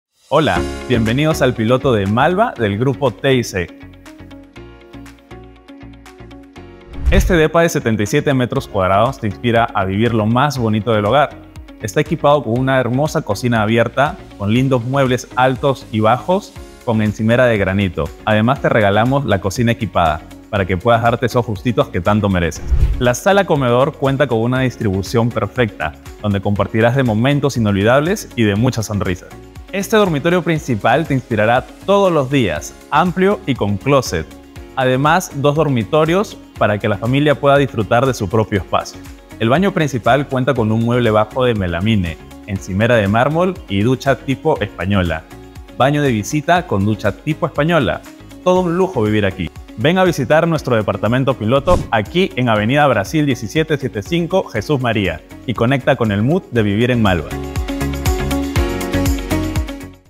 Conoce el depa piloto de Malva en Jesús María
Malva cuenta con una ubicación estrategica en Jesús María, sus departamentos cuentan con la calidez, buena distribución y excelentes acabados que te harán sacar tu lado más hogareño.
Departamentos de 1, 2 y 3 dormitorios en próxima entrega.
Más información en: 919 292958